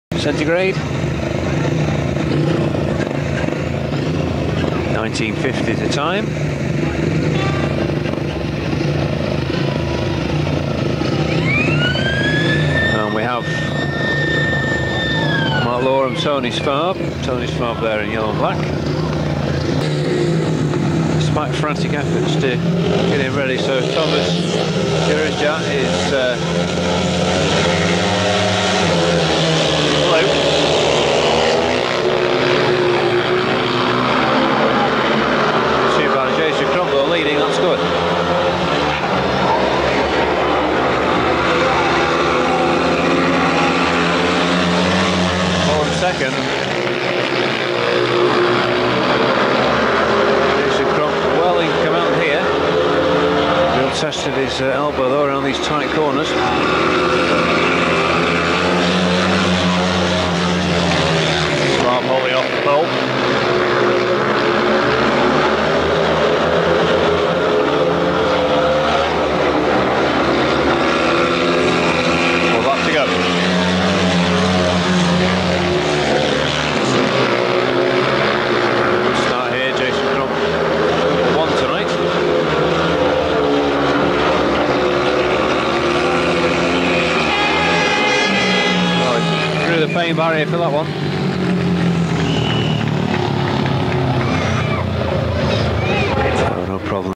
05.06.2002 Belle Vue v Eastbourne ELA H1 Speedway
Speedway: Belle Vue v Eastbourne 6th May 2002 (ELA) Heat 1
Red:  Jason Crump ,
Blue:  Matej Ferjan ,
White:  Mark Loram ,
Yellow:  Toni Svab ,
Aces on Video
This meeting is available from classicspeedwayvideos.com along with thousands of other vintage speedway videos from the 80s, 90s, and 00s..